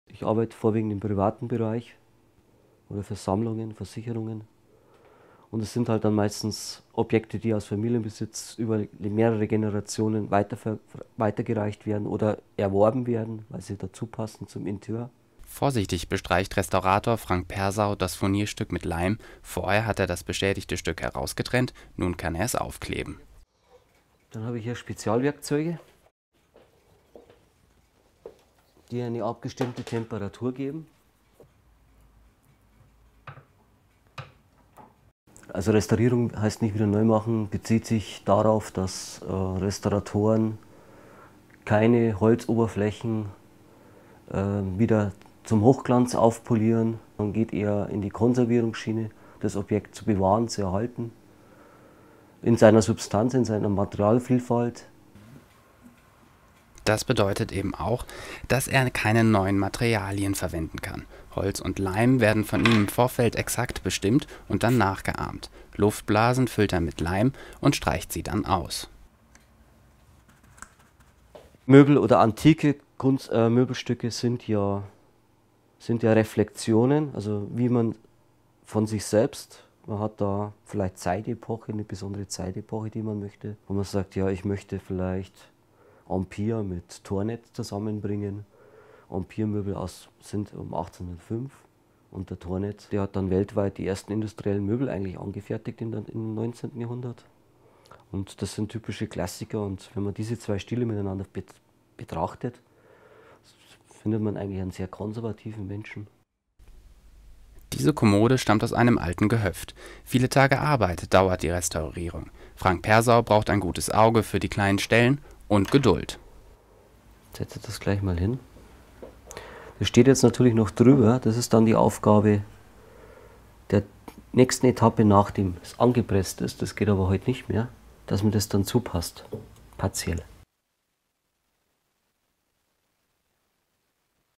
VJ-Beitrag Restaurator
Der Münchner Möbelrestaurator Frank Persau zeigt Handlungsschritte bei der Restaurierung einer Kommode und berichtet über seinen Werdegang.